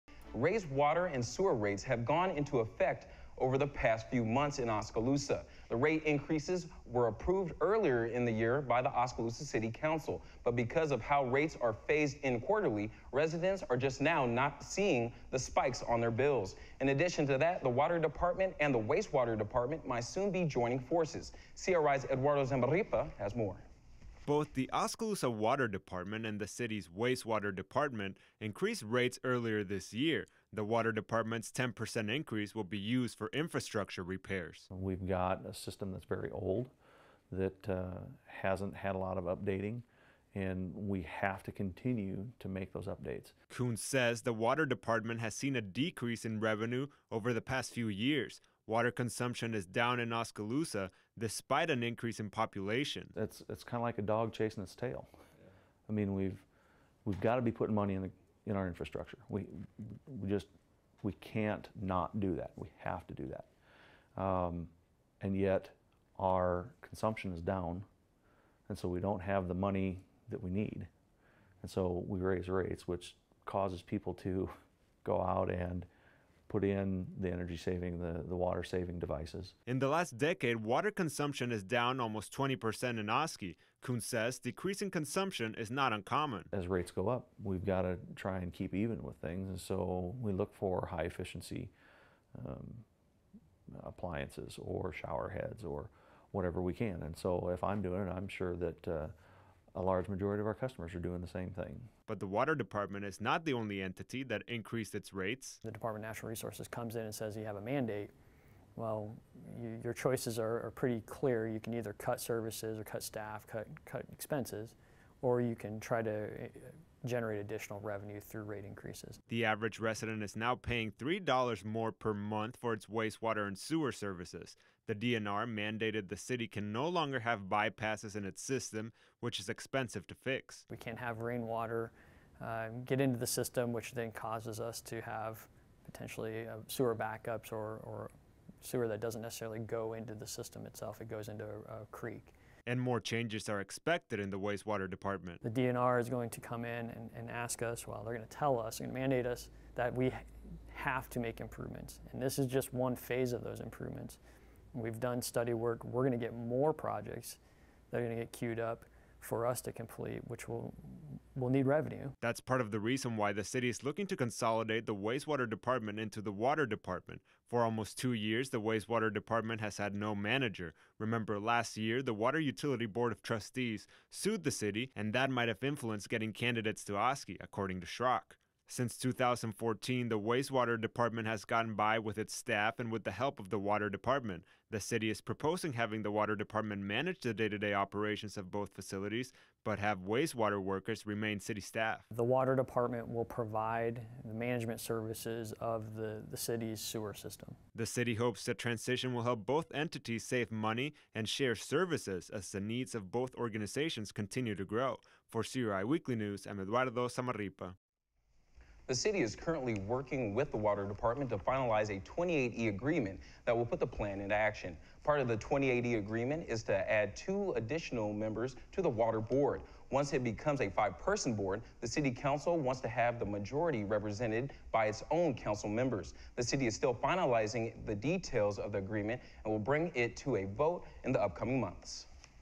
Water Department And Wastewater Looking To Consolidate With Rates Increasing
Raised water and sewer rates have gone into effect over the past few months in Oskaloosa. The rate increases were approved earlier in this year by the Oskaloosa City Council. But because of how rates are phased in quarterly, residents are just now noticing the spike on their bills. In addition to that, the water department and the wastewater department might soon be joining forces.